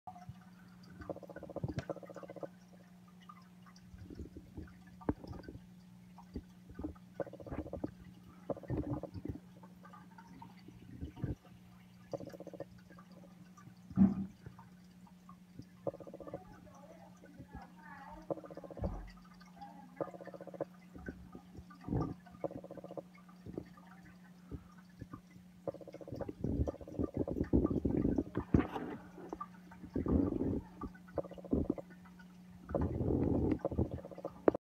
Feeding my Flower Horn dried shrimp
Feeding my flower horn dried shrimp he loves it